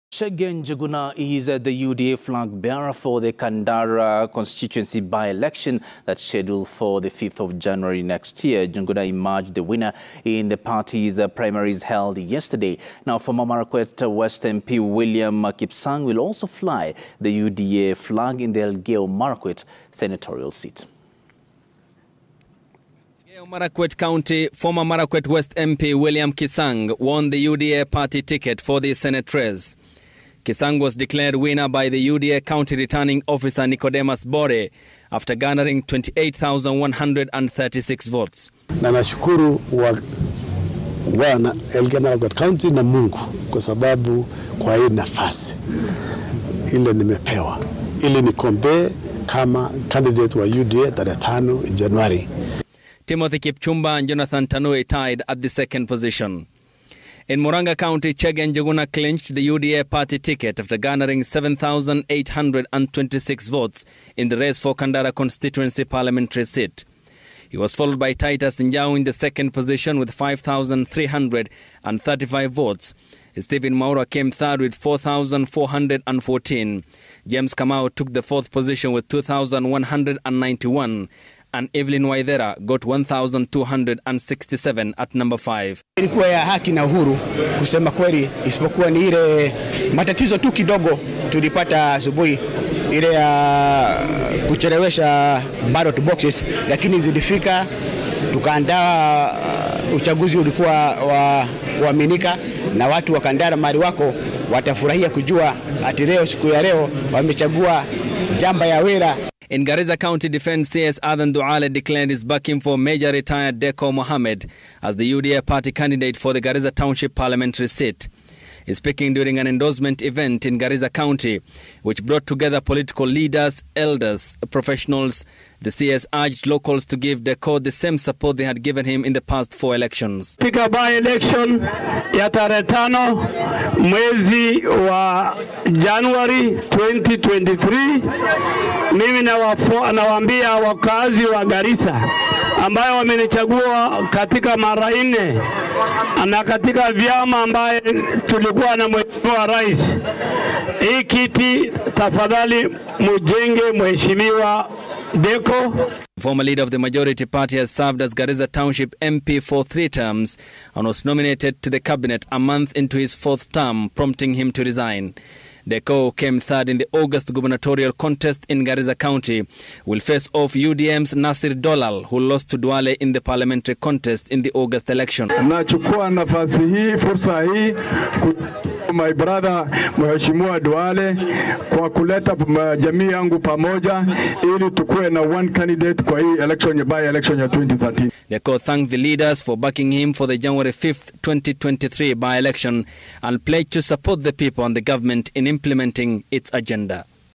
Chege Njuguna wins UDA ticket for Kandara MP
Chege Njuguna is the UDA flag bearer for the Kandara constituency by election. Njuguna emerged the winner in the party's primaries held yesterday. Former Marakwet West MP William Kipsang will also fly the UDA flag in the Elgeyo Marakwet senatorial contest.